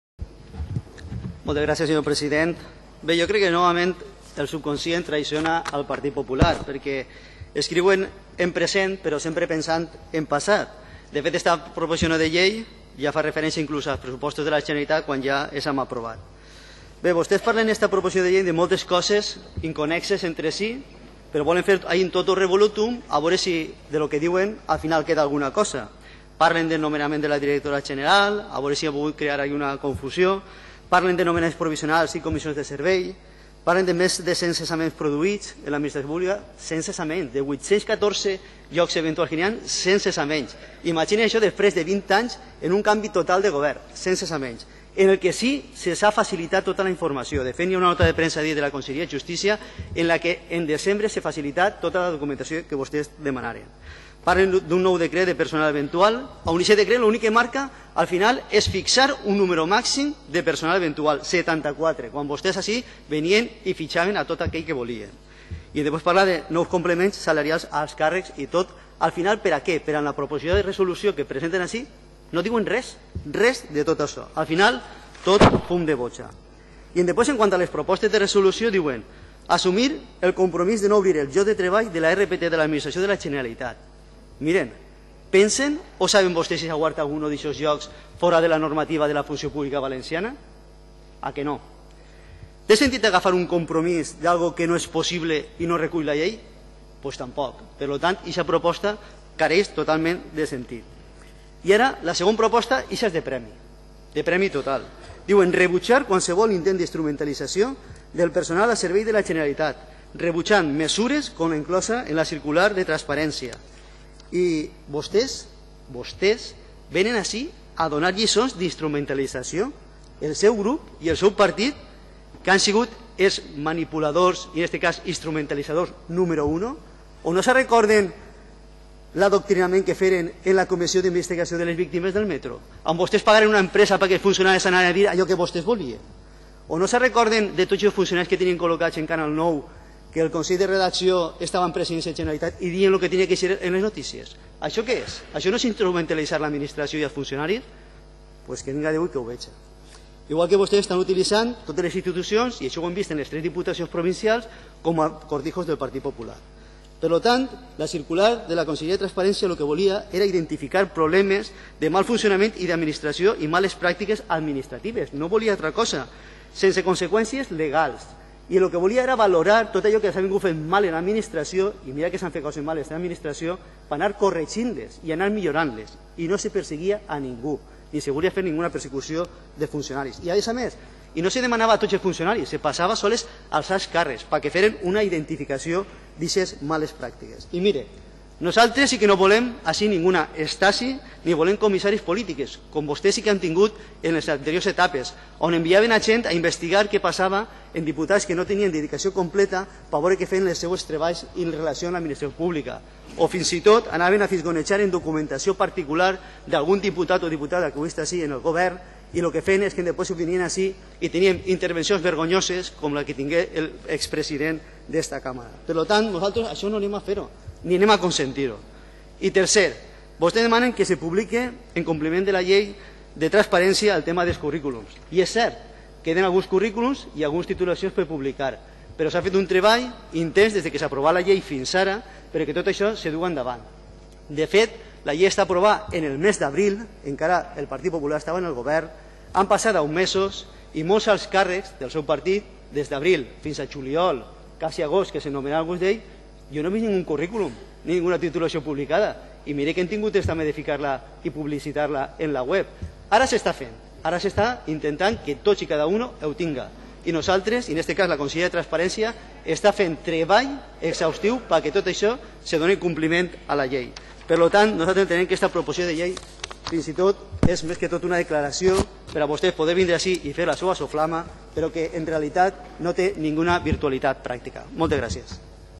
P. García - "Vénen a donar lliçons d'instrumentalització quan han sigut els més manipuladors"
Intervenció del diputat de Compromís, Paco García, el dimecres passat al ple de les Corts Valencianes sobre la dignificació i la millora de la funció pública valenciana